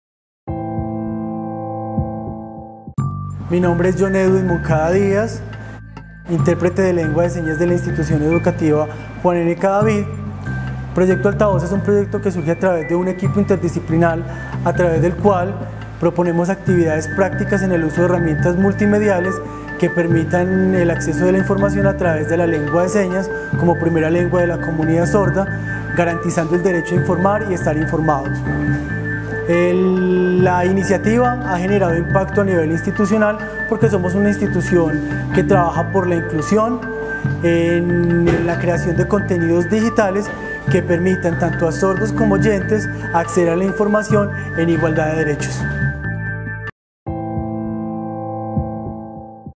John Edwin Moncada Díaz y Samuel Cardona
Estos maestros TESOS de la Institución Educativa Juan N. Cadavid piensan en todos sus estudiantes, sordos y oyentes, por eso lideran con ellos el proyecto Altavoz, a través del cual están produciendo contenidos informativos multimediales inclusivos.